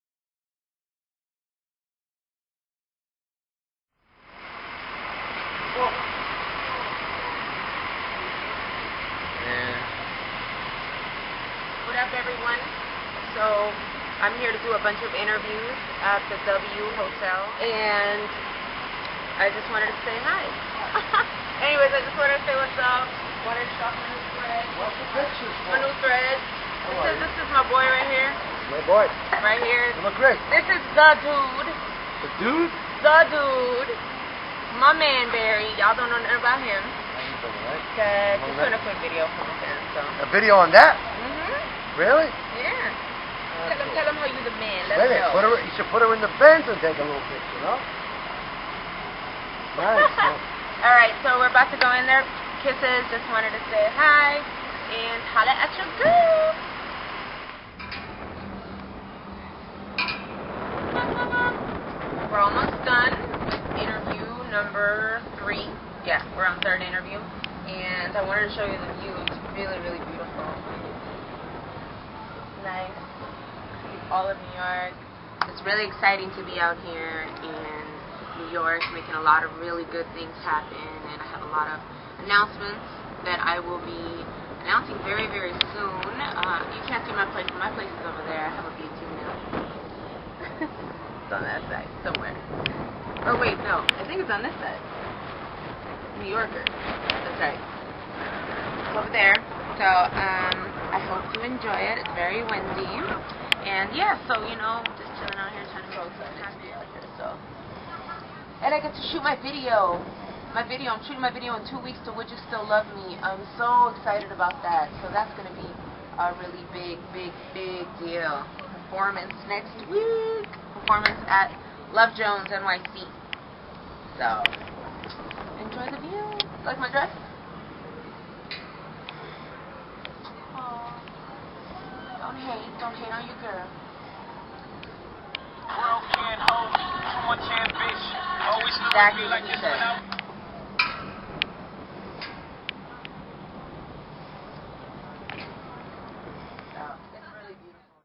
Somaya Reece's NYC experience blog #1 - The W Hotel!
Now that I moved to New york. I thought i'd share some moments with you.

I LOVE THE EAST COAST. My new home.